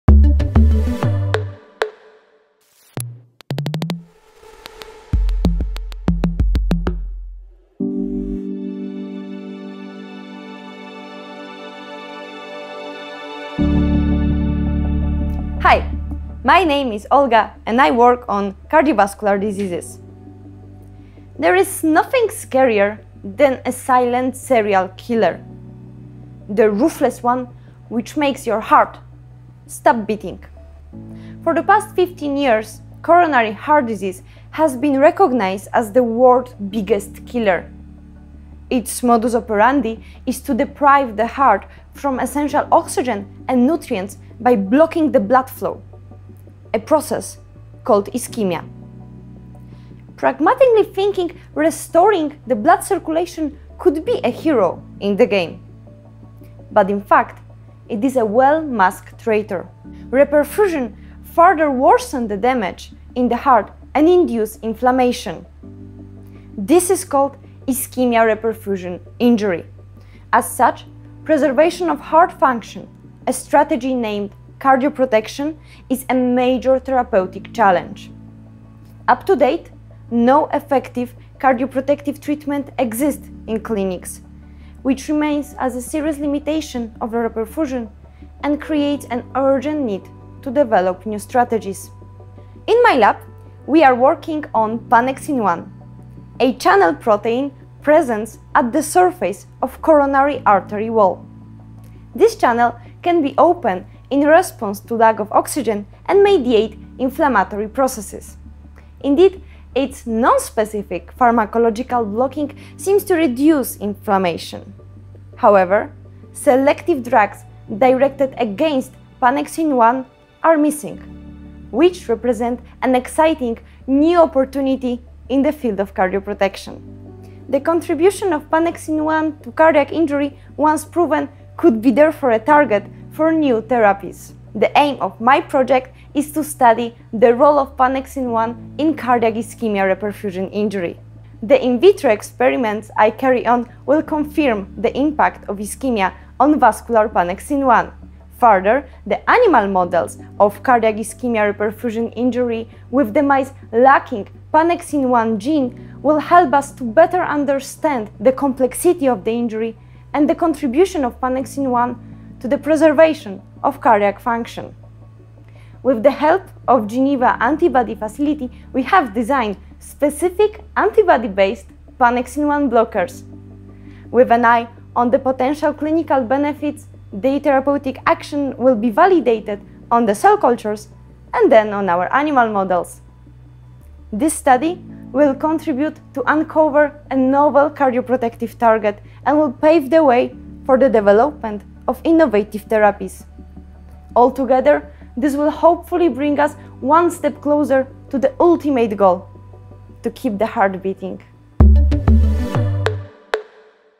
PhD Booster - Les bloqueurs de la pannexine 1: une course pour la vie
Olga Rusiecka est l'une des lauréates du projet "PhD Booster" lancé par la Faculté de médecine dont le but est de valoriser l’excellence de la recherche biomédicale à Genève en soutenant financièrement la carrière de cinq doctorants-es en sciences de la vie de l’Université de Genève.

Doctorante à la Faculté de médecine , Olga travaille sur les traitements potentiels pour lutter contre les maladies ischémiques.

Titre de thèse:
Les bloqueurs de la pannexine 1: une course pour la vie
Laboratoire: Prof. Brenda Kwak, Département de pathologie et immunologie,
Faculté de médecine, UNIGE
Thèse: octobre 2017 - mars 2022

Son projet:
Les cardiopathies ischémiques et l’insuffisance cardiaque ont non seulement un impact social important mais représentent également un fardeau économique. Mon projet pourrait ouvrir la voie à la découverte d’une nouvelle cible thérapeutique contre les cardiopathies ischémiques et l’insuffisance cardiaque, et contribuera à accroître les connaissances sur les voies de signalisation et les types de cellules impliquées. Le but de mon projet est d’étudier la contribution des canaux pannexine 1 (Panx1) aux lésions d’ischémie-reperfusion cardiaque. À l’aide de modèles de culture in vitro, nous étudions l’impact de l’ischémie sur la fonction endothéliale de la Panx1. Ceci permettra de mieux comprendre la complexité des lésions myocardiques et le rôle de la Panx1 dans la préservation de la fonction cardiaque afin de développer des inhibiteurs efficaces.